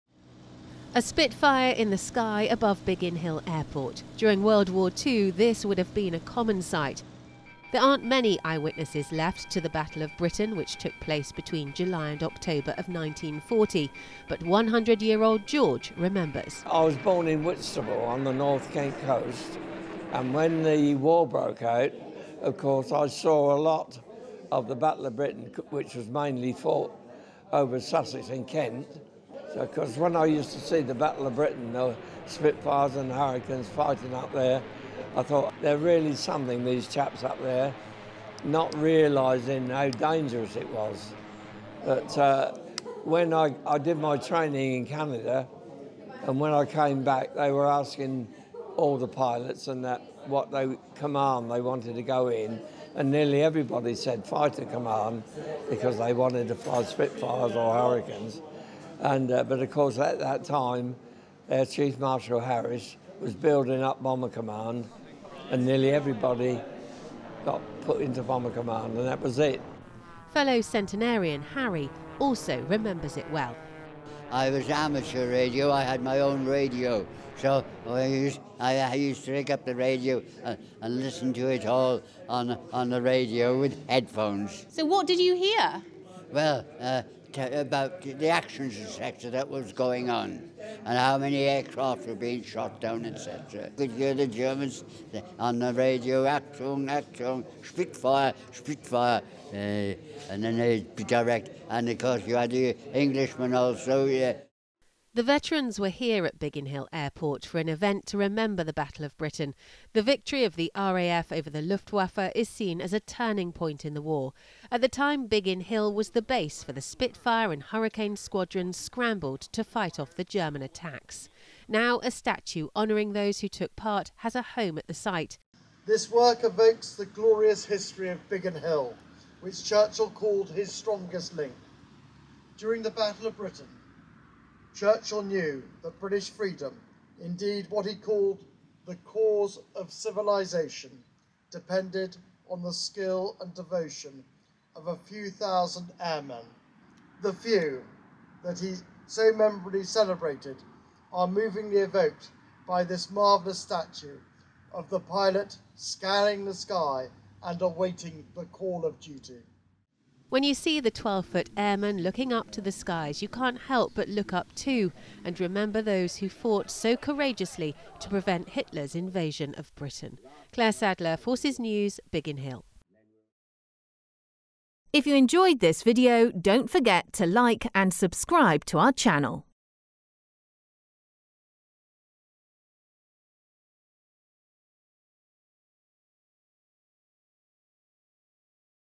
'The Few' honoured with a 12ft sky-gazing statue at Biggin Hill
Sir Winston Churchill's great-grandson has officially unveiled a sculpture of a Second World War pilot at Biggin Hill Airport, to commemorate the Battle of Britain.

World War Two veterans gathered with Randolph Churchill and the Mayor of Bromley at the former RAF station for an event to honour all who fought so courageously to prevent Hitler's invasion of Britain.

The Battle of Britain marked the victory of the RAF over the Luftwaffe and is seen as a crucial turning point in the war.